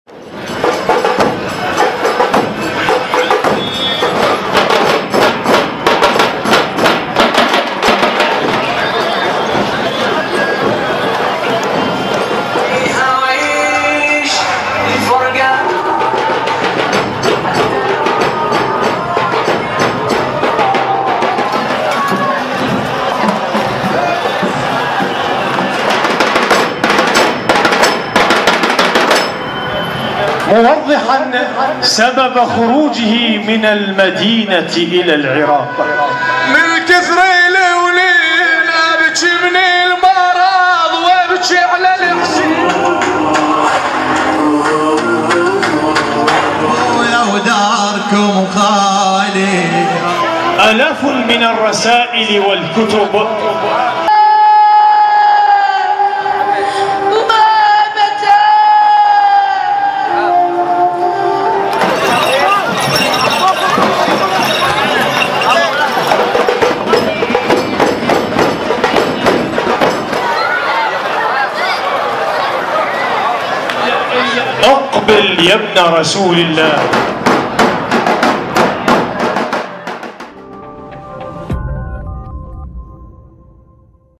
هل يحمل يزيد مبخرة في هذه التشابيه؟ راجمات ومنجنيق وألم عميق في “بلد”
9 شهور متواصلة من العمل الدؤوب والتواصل المستمر مع مخرجين وفنانين من العاصمة بغداد ومدن الجنوب ليخرج موكب الأنصار بمشاركة الكبار والصغار في شوارع بلد مستعرضاً واقعة الطف “التشابيه”، بأساليب فنية وإخراجية من ملابس وتجهيزات وإتقان دور الشخصيات التي ساهمت بإنجاح التدريبات الميدانية، بينما بدا “معسكر العدو” بشخصيات تحمل أشياء تشبه المباخر الاحتفالية، وقد ترمز إلى يزيد بن معاوية أو بعض جنرالاته من تلك الأيام.